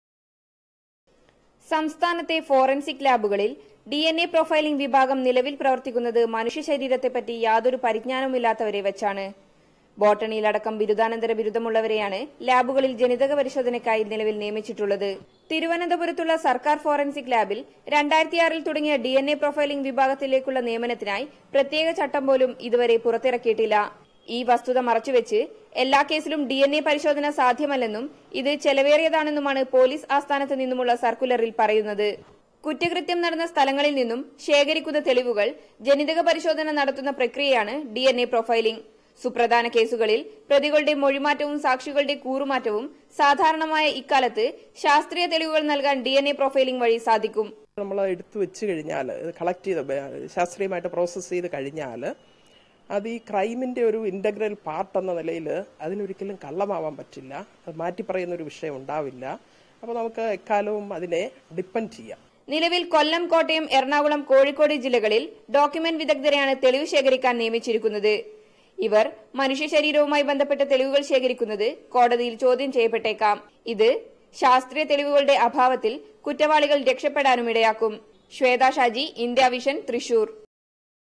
Lack of Forensic DNA Specialists in Kerala
സംസ്ഥാനത്ത് കുറ്റകൃത്യങ്ങളുടെ എണ്ണം വര്‍ദ്ധിക്കുമ്പോഴും ഡിഎന്‍എ പരിശോധനയ്ക്ക് വിദഗ്ധര്‍ ഇല്ലാത്തത് കേസുകളുടെ ശാസ്ത്രീയത തെളിവെടുപ്പിനെ ബാധിക്കുന്നു. മനുഷ്യശരീരത്തെ പറ്റി അടിസ്ഥാന പരിജ്ഞാനമില്ലാത്തവരാണ് ഫോറന്‍സിക് ലാബുകളില്‍ പരിശോധന നടത്തുന്നത്.